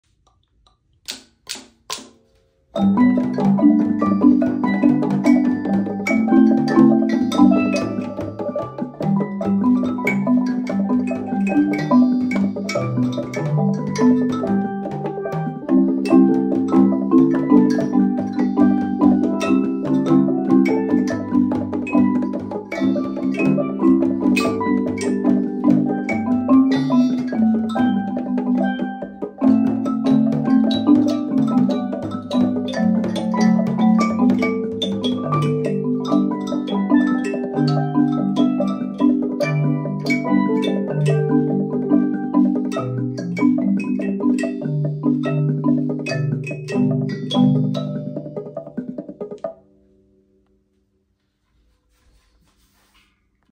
some of my crazy instruments, complete improv of each instrument Steelpan, marimba, Kalimba,Log drum
recorded each one as a solo instrument, one take at a time just to see what I could come up with when put together. Nothing planned just used the same key and tempo. Individual